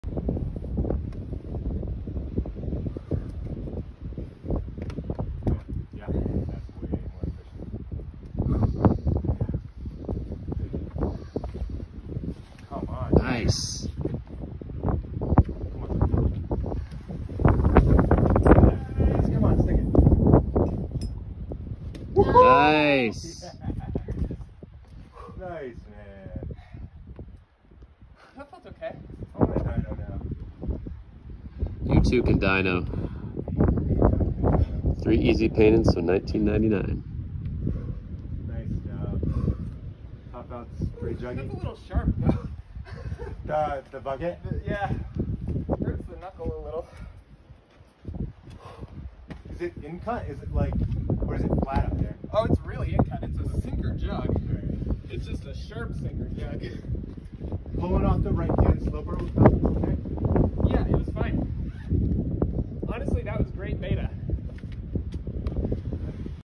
Ketron Classic (V4) | The Happies, Bishop
My flash of Ketron Classic (V4) in Bishop. And classic it is! Some stellar movement on this one combined with some neat holds. This is a must-try for anyone hitting up the Happies!